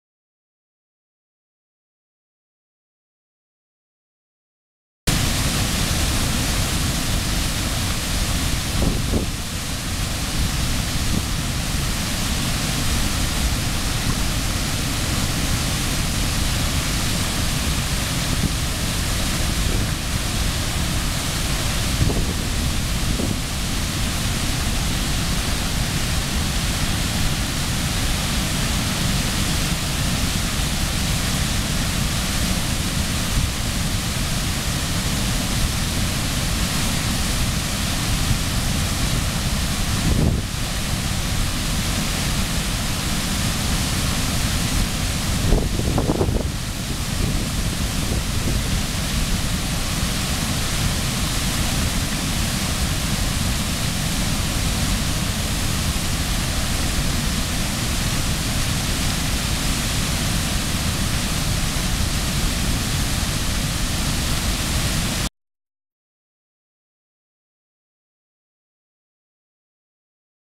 Hurricane Irene Manchaug Waterfall Sutton MA
This video was taken on August 28, 2011 at 3:10pm just after Hurricane Irene had finished its bombardment on Sutton Massachusetts. This is the Manchaug waterfall location across from the Manchaug Mill. The waterfall is brown in color from the debris it's swept up along its path. More photos at SuttonMass.org.